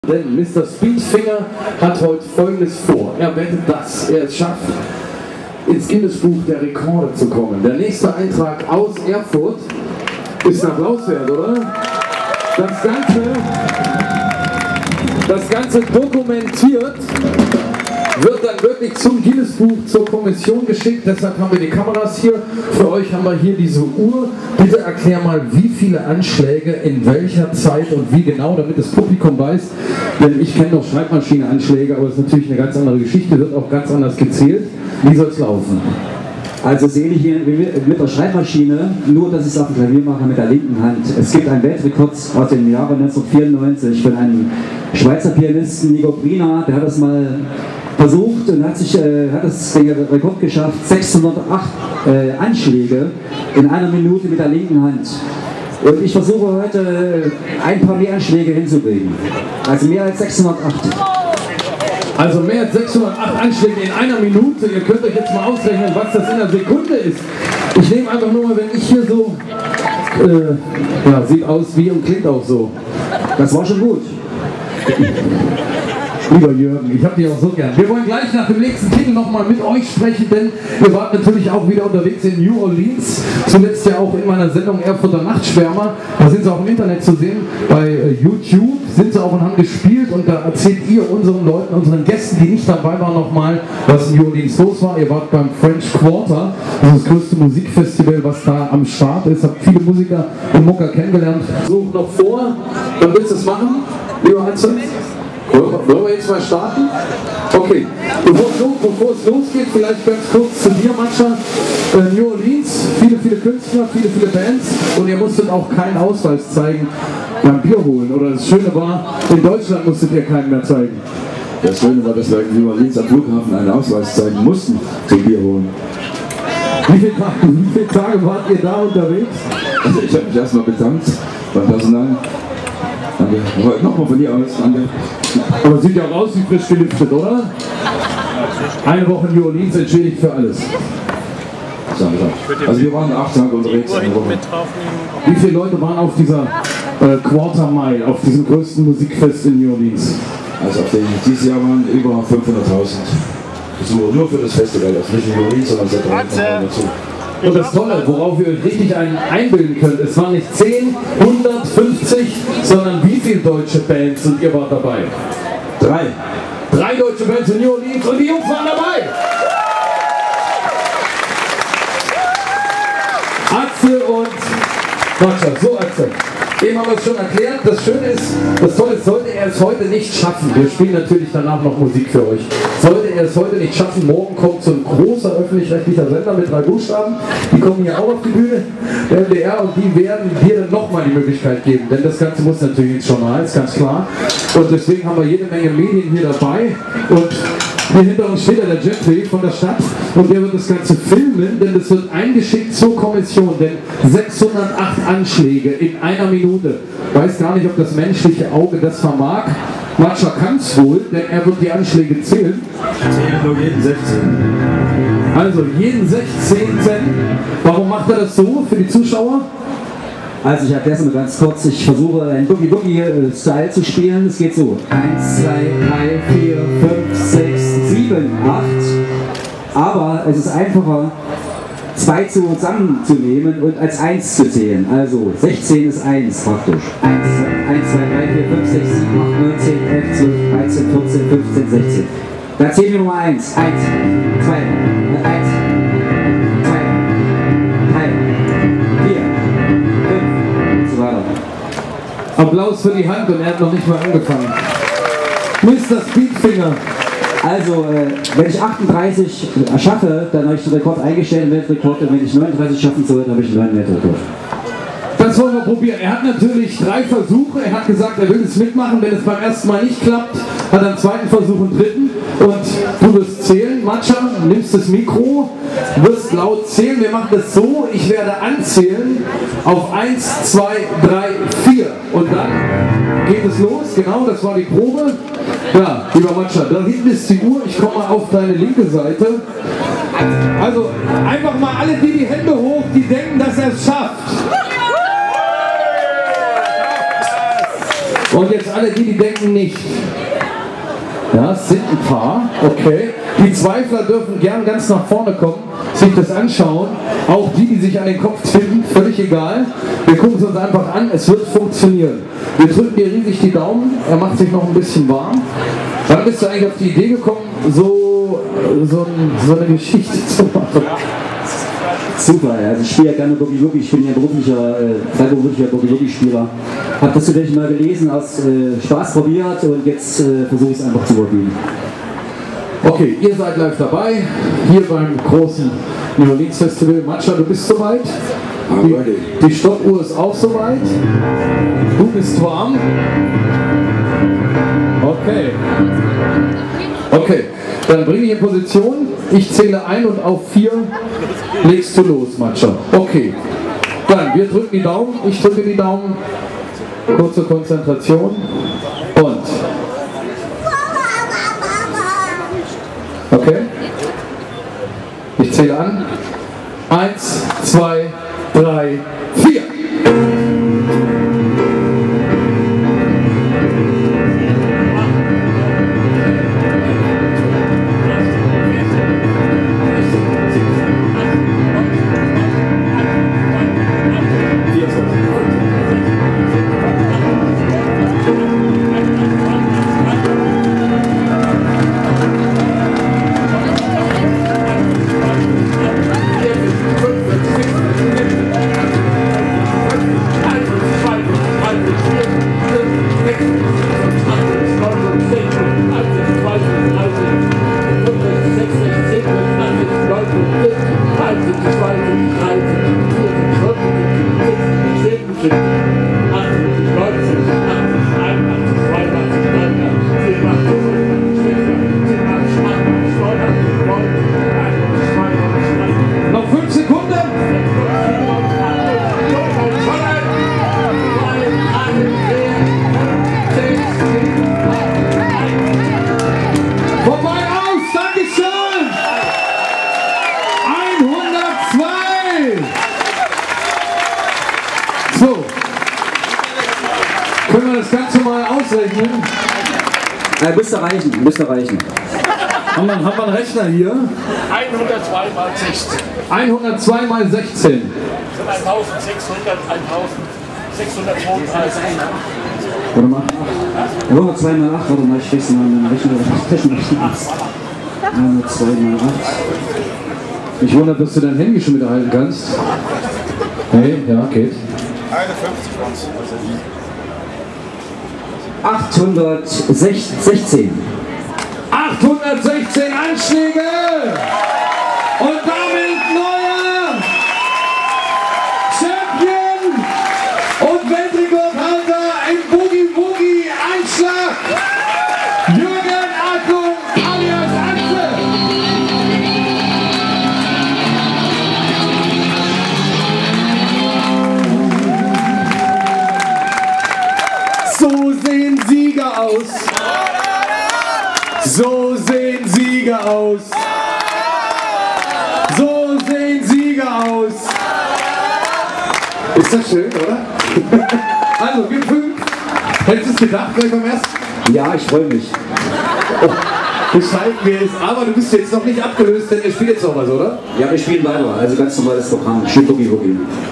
Weltrekord, Jürgen Adlung, schnellster Boogie-Pianospieler - New Orleans Festival Erfurt 2011
Ein Höhepunkt des diesjährigen New Orleans Music Festivals:
Atze Adlung hat es geschafft! Vor einem begeistert mitfiebernden Publikum  und einer amtlich präzisen Zeitmessanlage hat er sich seinen Platz im Guinnes Buch der Rekorde souverän erspielt. Er lag sogar noch weit über den geforderten Tastenanschlägen in der festgelegten Zeit.
Atze ist somit der schnellste Boogiepianospieler der Welt.
Bleibt nur zu hoffen, daß sich das auch für ihn und seinen Musikerkollegen Matscher von Boogieman`s Friend in klingender Münze bemerkbar macht.